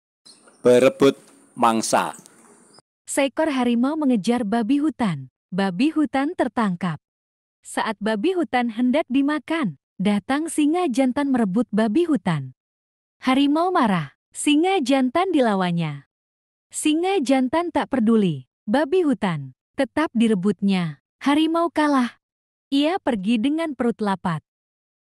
Berebut Mangsa
Seekor harimau mengejar babi hutan. Babi hutan berhasil dia tangkap. Ketika hendak dimakannya, datang Singa jantan. Singa merebut babi hutan dari harimau. Tentu harimau marah. Dipertahankan mangsa buruannya itu sekuat tenaga.
Namun dalam perebutan tersebut, harimau kalah. Babi hutan dibawa lari sang singa. Maka harimau pergi dengan perut yang masih kosong. Lapar.